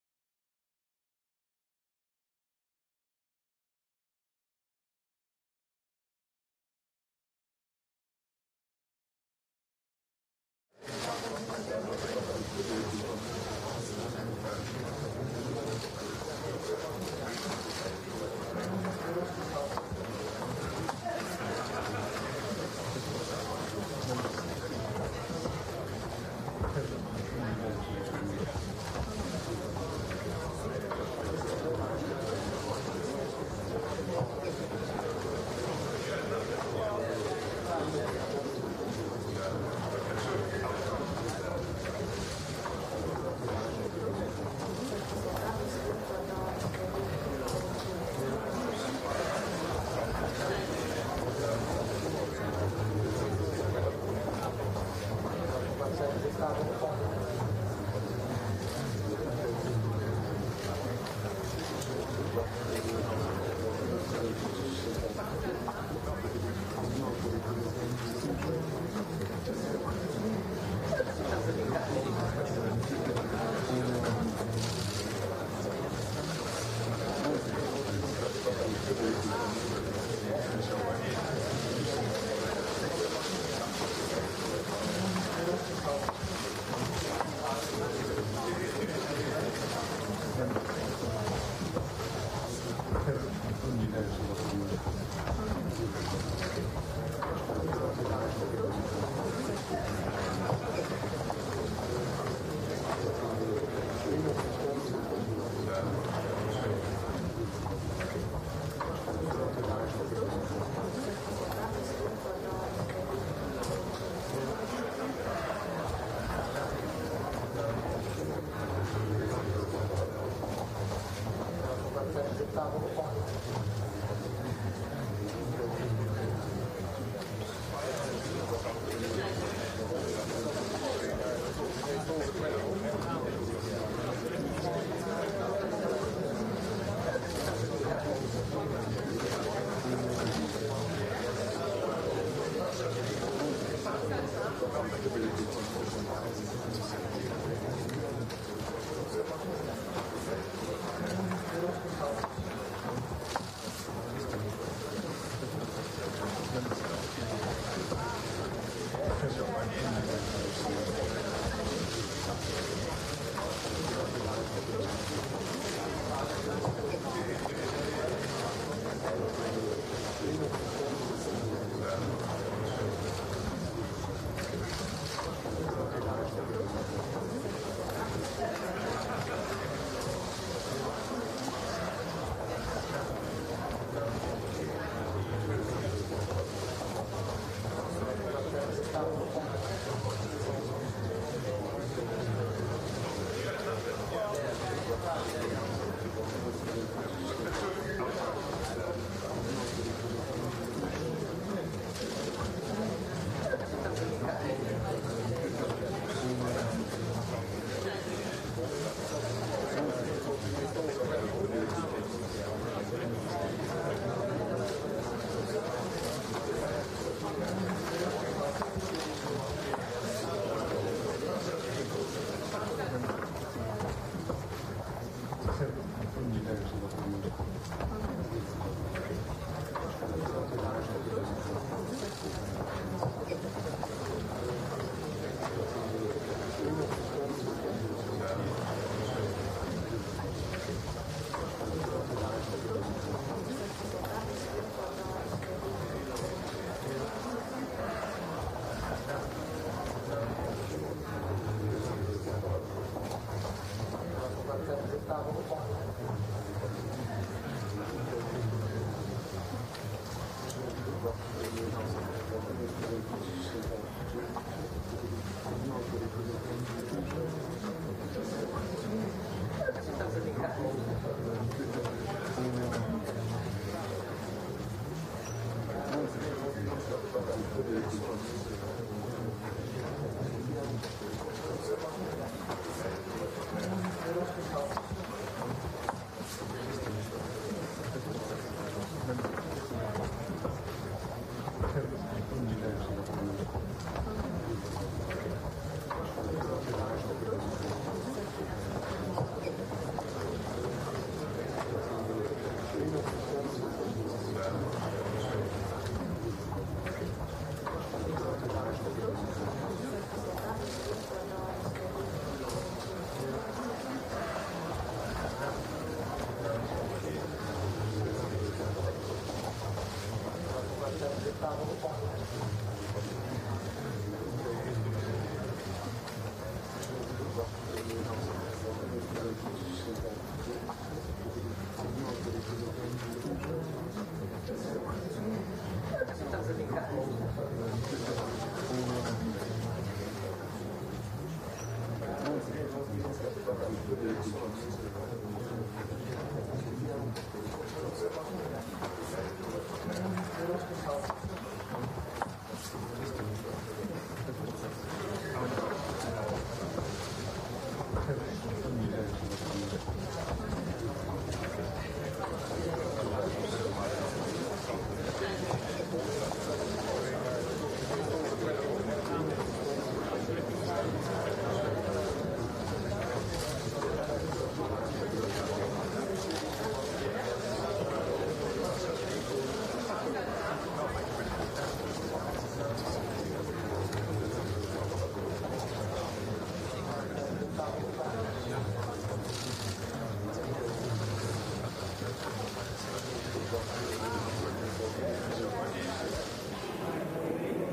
16.10.2020 - Angela Merkel und alle anderen (2. Tag) - Europäischer Rat
Schnittbilder der Bundeskanzlerin und der anderen Staats- und Regierungschefs der Europäischen Union am ersten Tage vor Beginn der Sitzung zum Europäischen Rat in Brüssel vom 15. Oktober 2020, 9.55 Uhr / Kanzlerin ab 00:03:15 (9.59 Uhr)